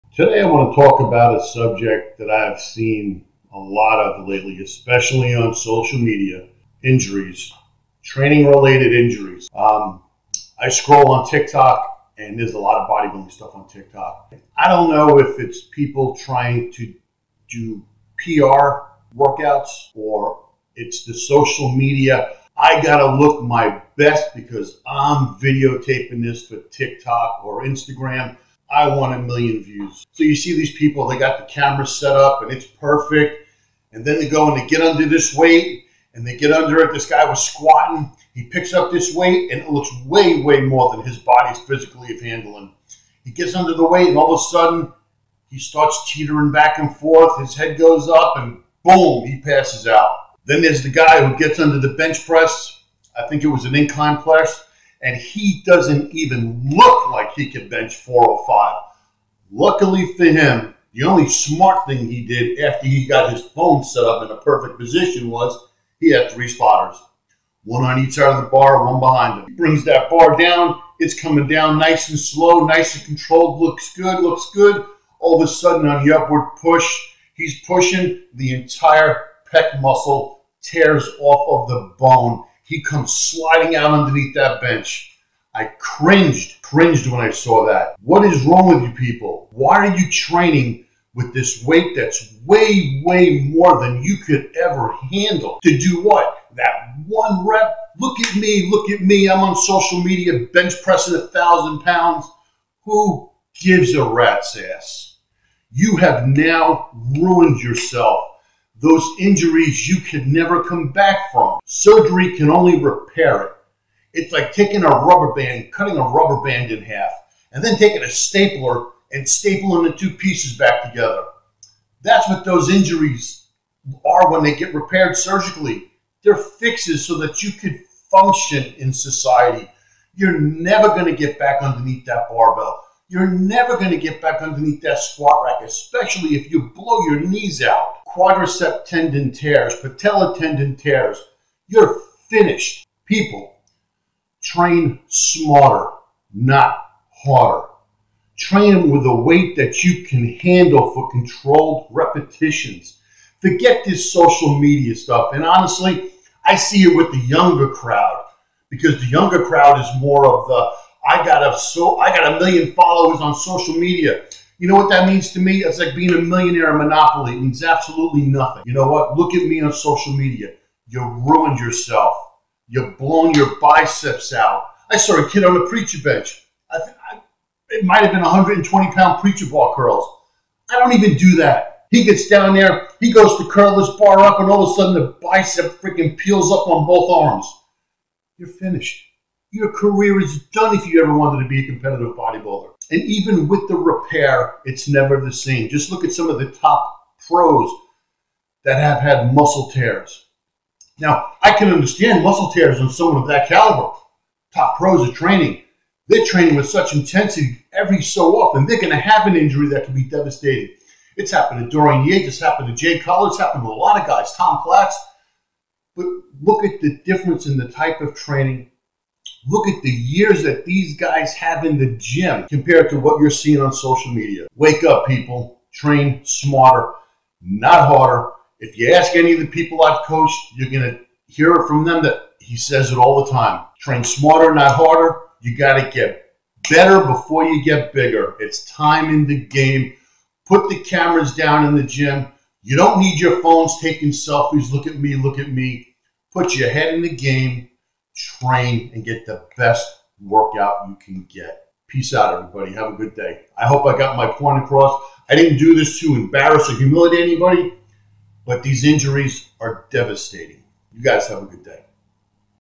Injury Free Bodybuilding - No Ego Training
Looking to transform your body without risking injury? 🏋️‍♂️ Dive into Injury Free Bodybuilding in the gym. Focus on Proper  Form First, Forget the Fame," where Coach Richard Politano, with years of fitness expertise, especially for those over 40, reveals how to train smarter, not harder. Discover the secrets to proper muscle engagement and see results by incorporating easy exercises that grow your muscles fast without the unnecessary risks seen on social media.

This video is a goldmine for anyone eager to understand how to build a strong, injury-free physique by focusing on technique over temporary fame.

From the dangers of chasing viral fame with unrealistic weights to the importance of patience and perseverance in bodybuilding, Coach Politano shares eye-opening stories and practical tips that will change the way you approach your workouts. Learn the quick and easy way to make significant gains while protecting your body. Whether you're aiming for rear delts of steel or simply want to enhance your overall workout effectiveness, this video is packed with wisdom to guide you towards success and avoiding injuries.

Remember, it's about getting better before getting bigger and proper form first. So, put down your phone, focus on your form, and let Coach Politano lead the way to a stronger, safer bodybuilding journey that's injury free.

Your journey to a more robust, injury-free body starts now!

⭕CHAPTERS:
0:00 - Intro
0:30 - TikTok Squat Fail
0:50 - Incline Bench Press Fail
1:25 - Train Smarter Not Harder

⭕ 𝐓𝐑𝐀𝐈𝐍𝐈𝐍𝐆 𝐂𝐎𝐔𝐑𝐒𝐄𝐒